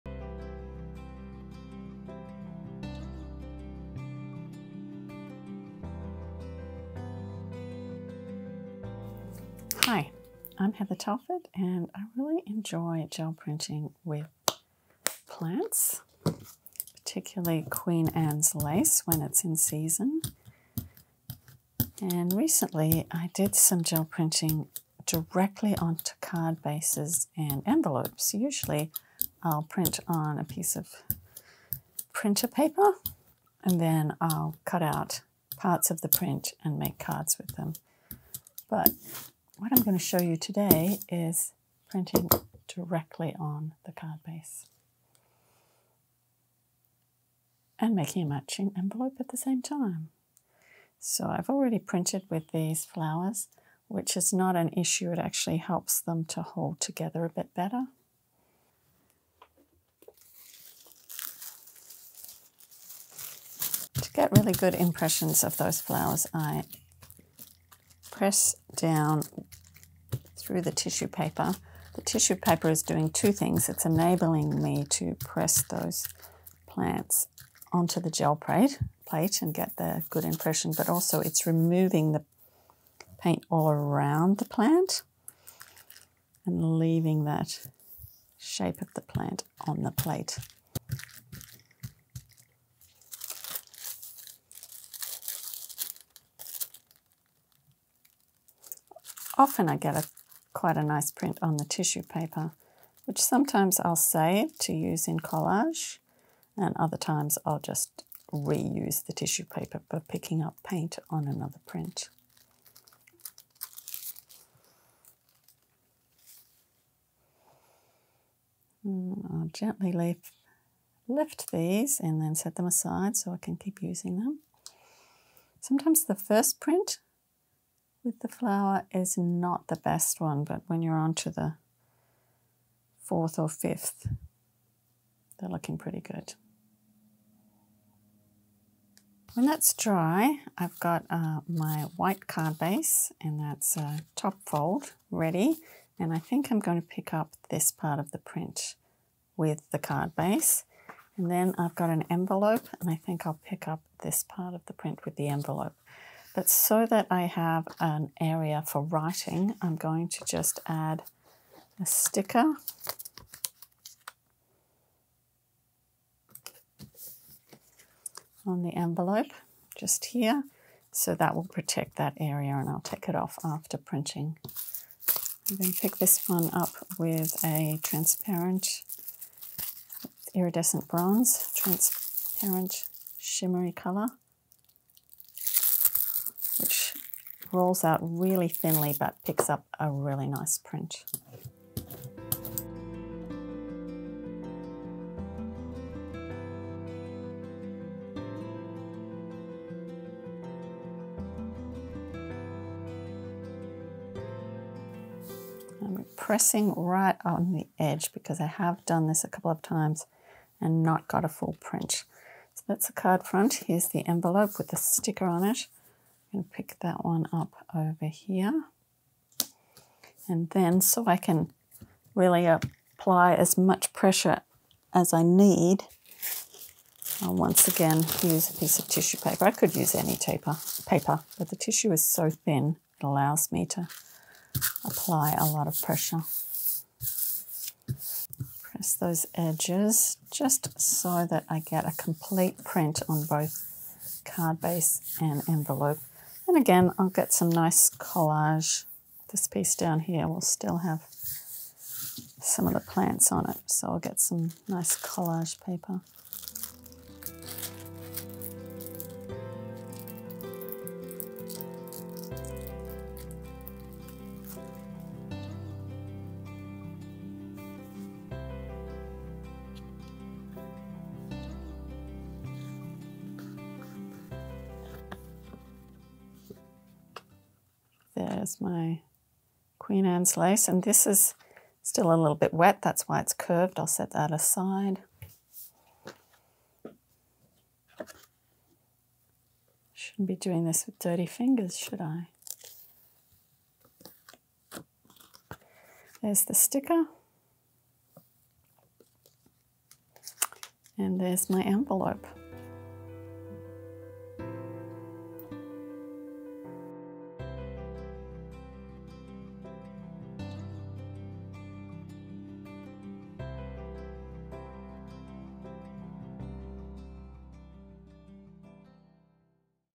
Floral Gel Printed Card + Envelope Combo
One of my favourite things to gel print is plants so printing a matching floral card and envelope seemed like a good idea.

If you are new to gel printing check out my online class Gel Print Journey which you can find

Supplies used for this technique
9"x11" gel plate - Gel Press
Liquitex Prussian blue hue paint
4" brayer - Speedball
iridescent bronze acrylic paint - Sennelier
Neenah solar white classic crest cardstock
Mohawk white envelope